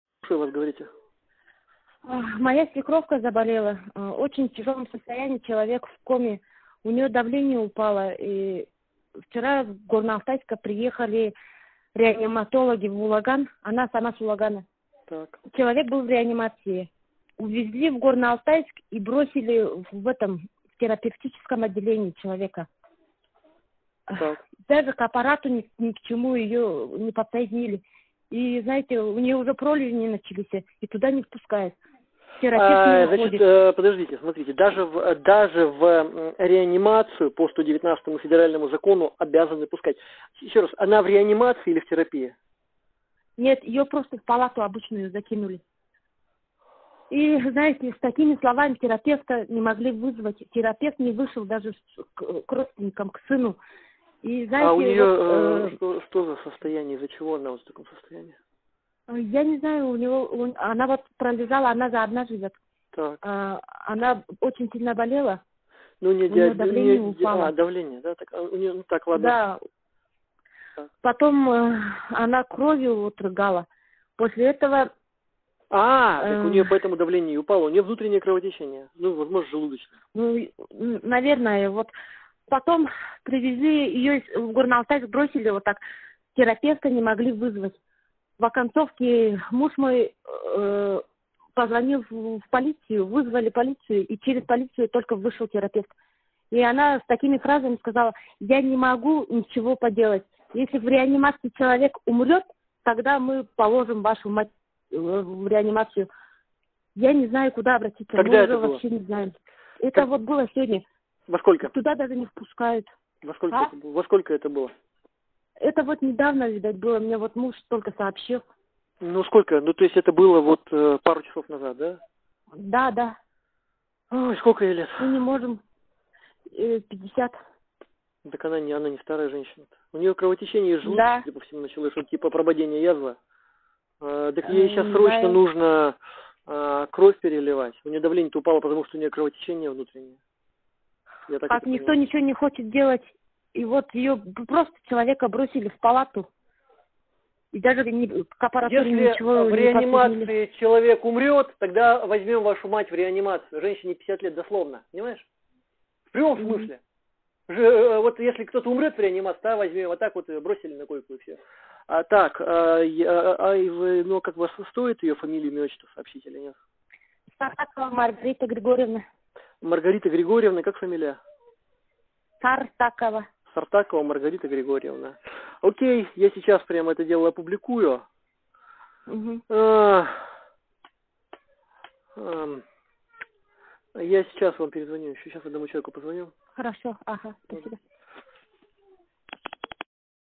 "Если в реанимации человек умрёт, тогда положим вашу мать в реанимацию"
50-летнюю женщину, у которой упало давление, которую рвало кровью, и которая находится в коме, в ресбольнице не стали класть в реанимацию. Терапевт соизволила поговорить с сыном пациентки только после того, как он вызвал полицию. А про перспективы направления пациентки в реанимацию объяснили просто: мол, ждите, когда там кто-нибудь умрёт, тогда место для вашей матери освободится. Такую историю только что рассказала по телефону сноха Маргариты Сартаковой из Улаганского района.

Сама пациентка - на фото, на которое мы наложили звукозапись разговора. Судя по фотографии, хотя бы капельницу Сартаковой поставили - какая-то медицинская помощь всё же оказывается.

 Сноха: - Моя свекровь заболела. В очень тяжелом состояние, человек в коме. У неё давление упало и вчера из Горно–Алтайска приехали реаниматологи в Улаган. Она сама с Улагана. Человек был в реанимации. Увезли в Горно-Алтайск и бросили в терапевтическом отделение человека. Даже к аппарату, ни к чему её не подсоединили. И знаете, у нее уже пролежни начались, и туда не пускают.

Сергей Михайлов: - Даже в реанимацию по 119-му федеральному закону, обязаны пускать. Еще раз, она в реанимации или в терапии?

С.: - Нет, её просто в палату обычную "закинули". «И ожидайте» - с такими словами терапевта не могли вызвать. Терапевт не вышел даже к родственникам, сыну.

С.М. :- А у нее что за состояние? Из-за чего она вот в таком состояние?

С.: - Я не знаю. Она же одна живёт. Она очень сильно болела.

С.М.: - У нее диабет?

С.: - Давление. Потом она вот кровью рыгала.

С.М.: - А, у нее поэтому давление и упало. У нее внутреннее кровотечение, возможно желудочное?

С.: - Наверное. Потом, привезли ее в Горно-Алтайск, бросили вот так, терапевта не могли вызвать. В концовке муж мой позвонил в полицию, вызвали полицию. Через полицию только вышел терапевт. И такими фразами сказала: «Я не могу ничего поделать, если в реанимации человек умрет, тогда мы положим вашу мать в реанимацию». Я не знаю куда обратится.

С.М.: - Когда это было?

С.: - Это вот было сегодня.

С.М.: - Во сколько?

С.: - Это вот недавно было. Мне вот муж только сообщил.

С.М.: - Во сколько? Это было пару часов назад?

С.: - Да, да.

С.М.: - Сколько ей лет?

С.: - 50.

С.М.: - Так она не старая женщина. У нее кровотечение в желудке началось, типа прободение язвы? Так ей сейчас срочно нужно кровь переливать. У нее давление-то, похоже. упало из-за кровотечения внутреннего.

С.: - Так никто ничего не хочет делать. И вот ее просто, человека, бросили в палату. И даже к аппарату не подключили.

С.М.: - Так, а стоит ее имя, фамилию, отчество сообщить?

С.:- Сартакова Маргарита Григорьевна.